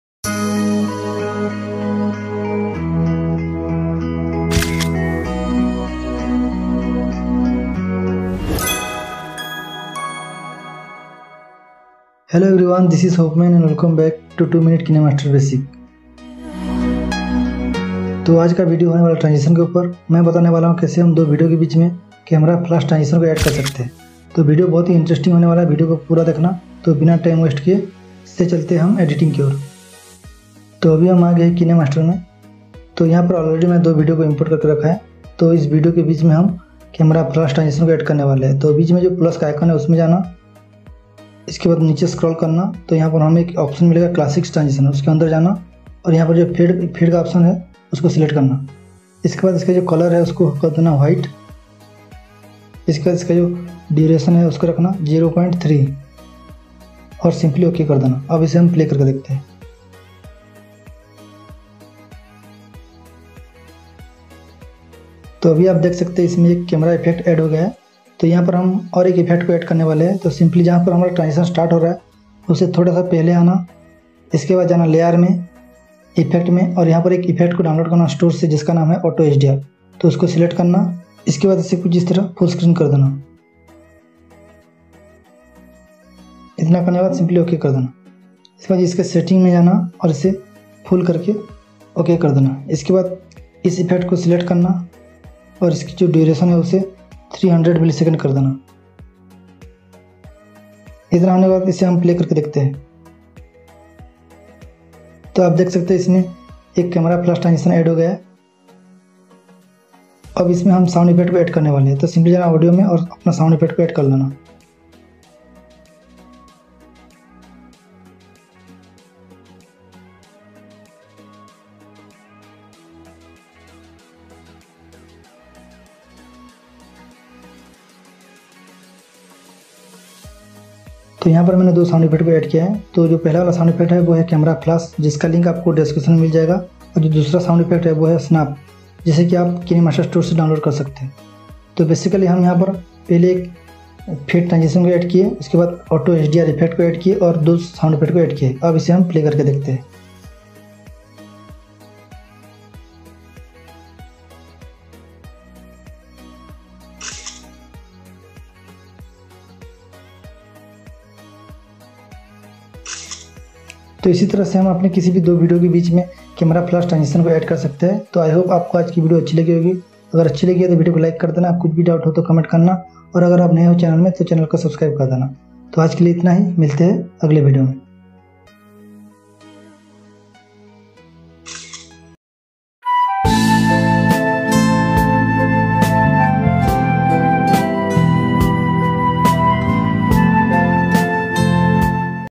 Camera Flash Transition Kinemaster | Kinemaster Video Editing
Learn how add camera flash transition in between two videousing Kinemaster video editor.